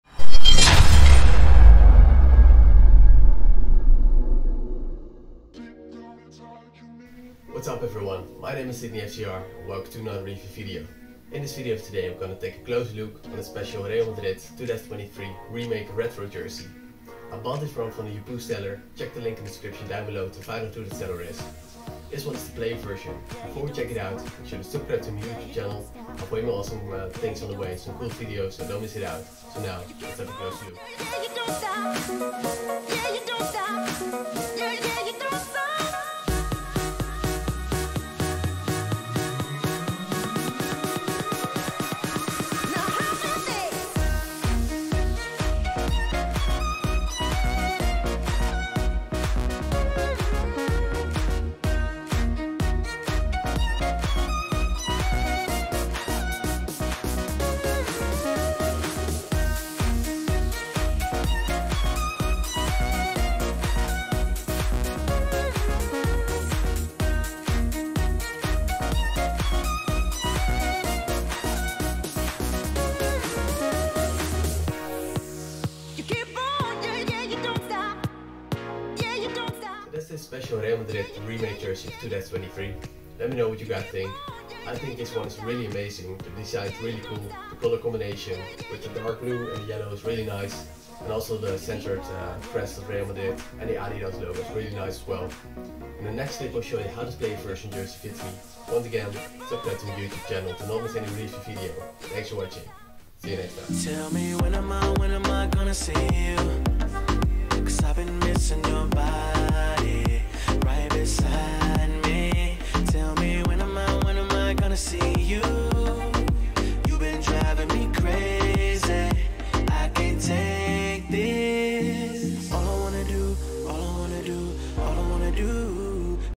Adidas Real Madrid 2023 Icon Retro Remake Jersey Unboxing [Dengjingfeng]
Hi! In this video I will review this special ADIDAS Real Madrid 2023 Retro Icon Remake shirt. Watch till the end to see how this jersey fits me!

✔️ Watch this video in 4K resolution!

🕘 Timecodes
0:00​​​​​ - Logo intro
00:07​​​​​ - Introduction
00:28 - Jersey review
01:53​ - Outro
02:19​​​​ - Jersey try on
02:35​​​​​ - End screen

💬 Let me know what you think of this Real Madrid fan version jersey! madrid​​ Adidasfootball​​​​ Jerseyreview​​ SidneyFCR​​

❌ I do not encourage ordering fake jerseys, just reviewing them for entertaining and educative purposes!

(Purchases at own risk!)

👕 I have the Adidas player version size S.
My specs: 175cm tall | 55kg ±